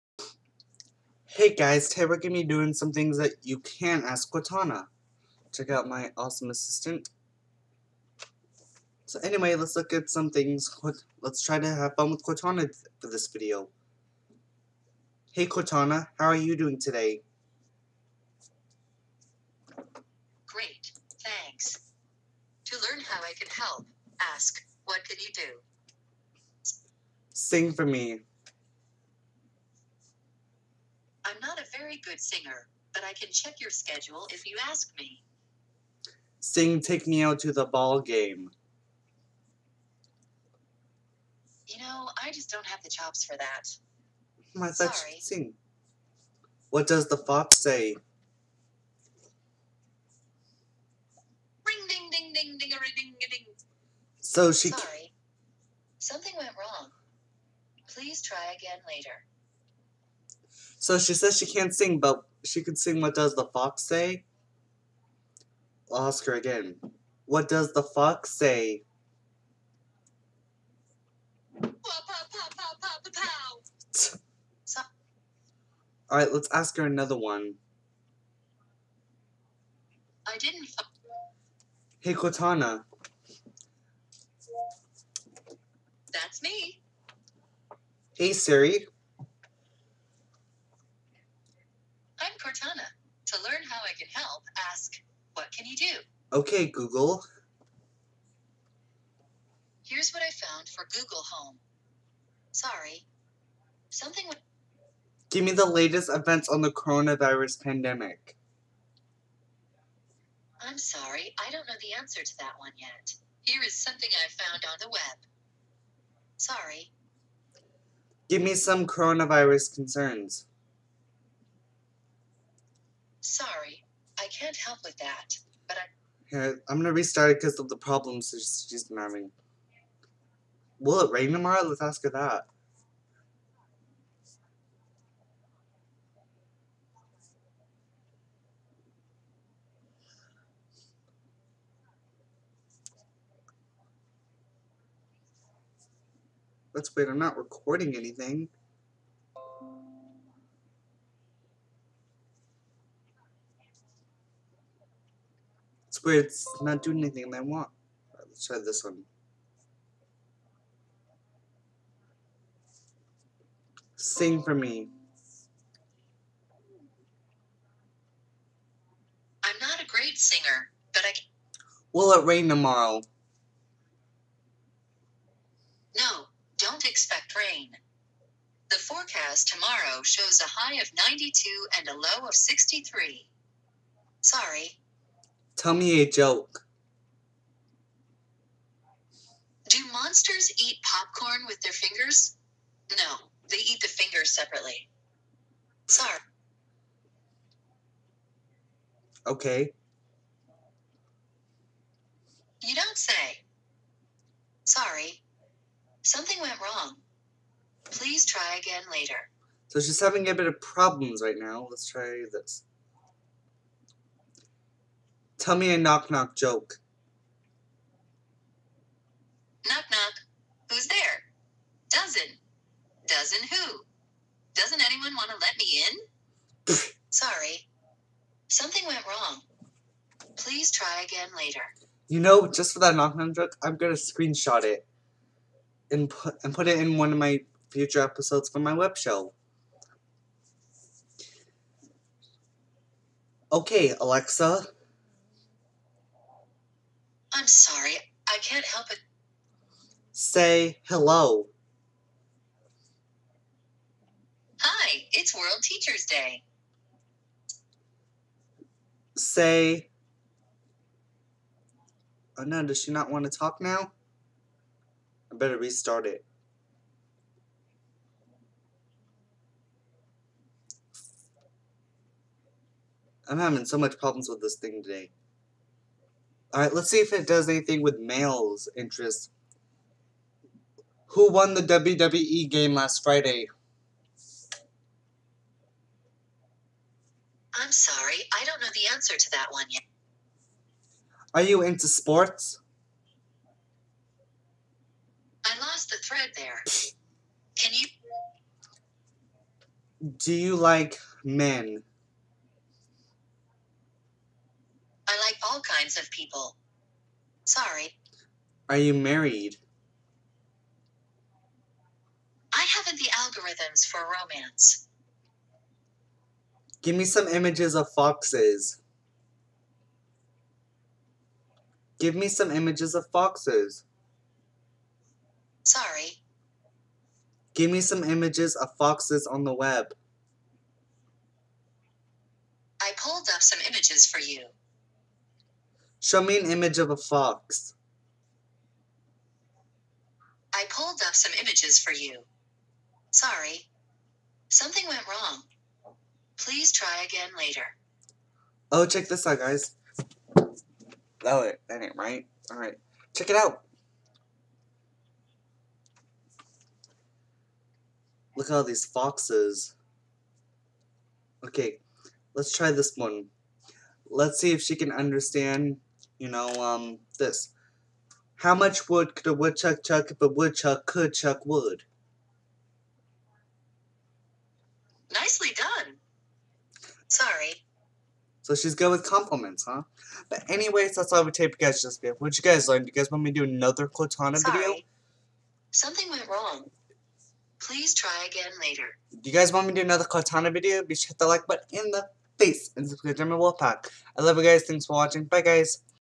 A chill day with Cortana
I'm reacting to some cool things you should and shouldn't ask Cortana.